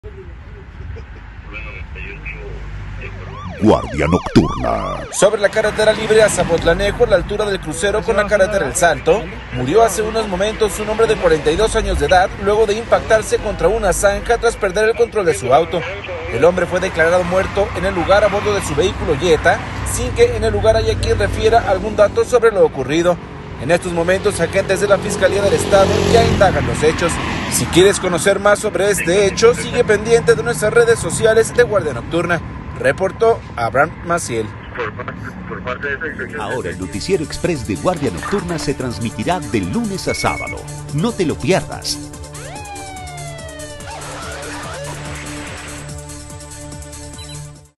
Un hombre murió tras impactar su vehículo contra una zanja ubicada sobre carretera libre a Zapotla
Preliminar Un hombre murió tras impactar su vehículo contra una zanja ubicada sobre carretera libre a Zapotlanejo, a la altura del crucero a El Salto Guardia Nocturna